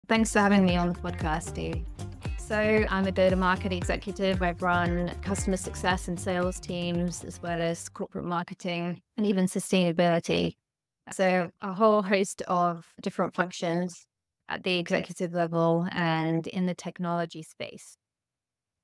Episode 17 - Introduction to Neelam Sandhu
In this 00:19 video, Neelam Sandhu, Former Chief Elite Customer Officer, Chief Marketing Officer, & SVP Sustainability, shares insights on transforming sales strategies to focus on customer success. She discusses aligning sales with customer-centric approaches, the impact of technology on relationships, and key strategies for long-term engagement. Prioritizing customer success fosters sustainable growth.